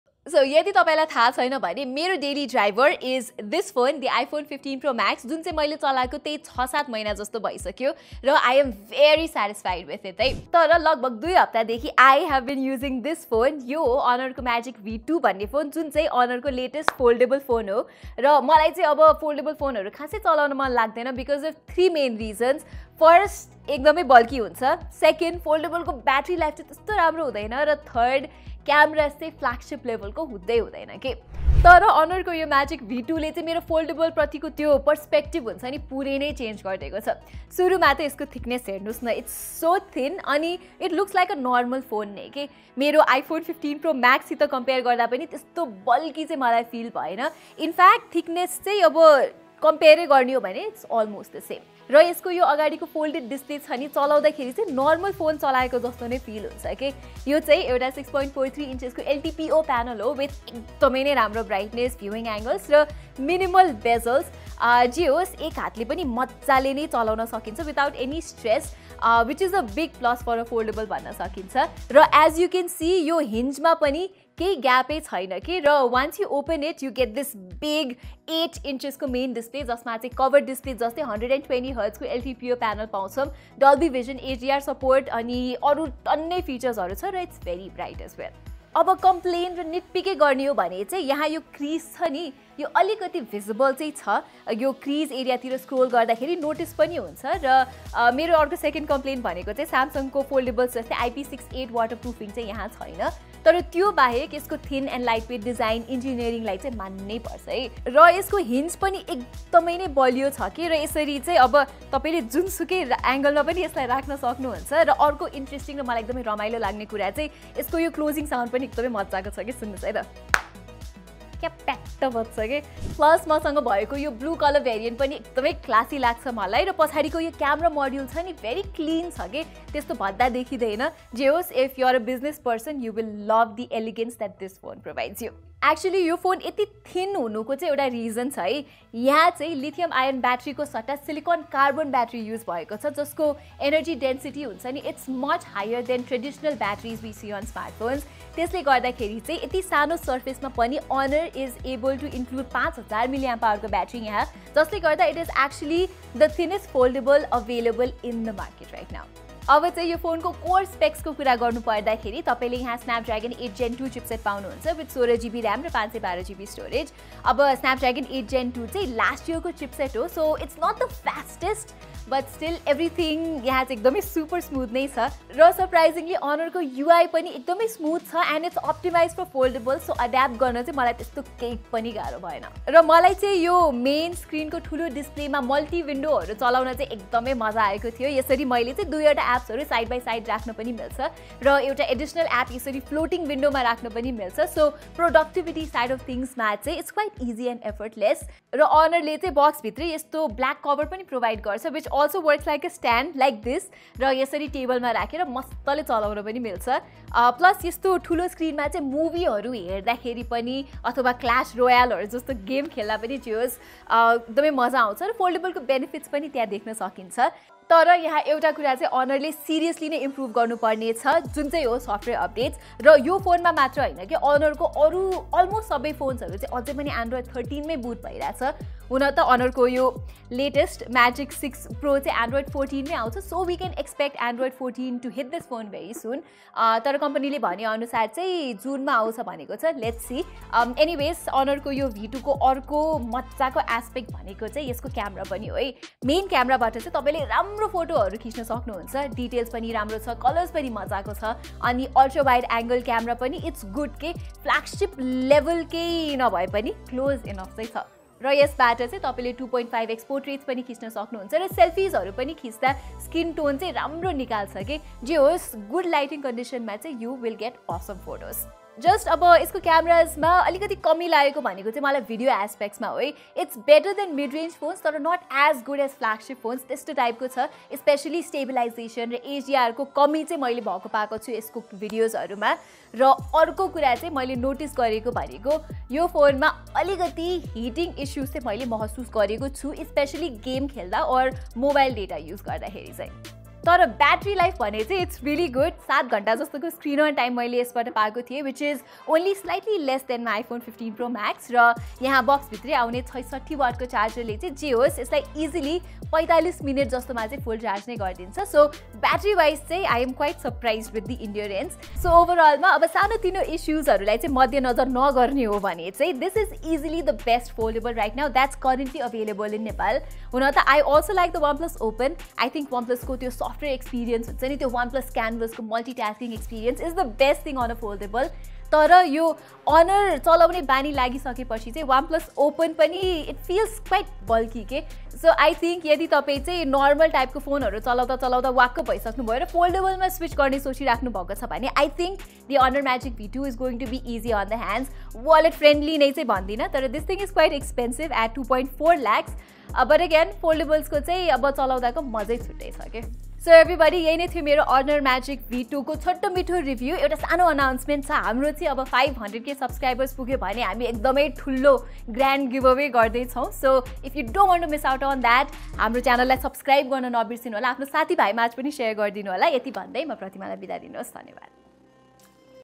I Used World's Thinnest Foldable! - Honor Magic V2
I have been reviewing the new Honor Magic V2 foldable phone for a week and here's my full review of it.

(Freebies up to Rs. 40,000)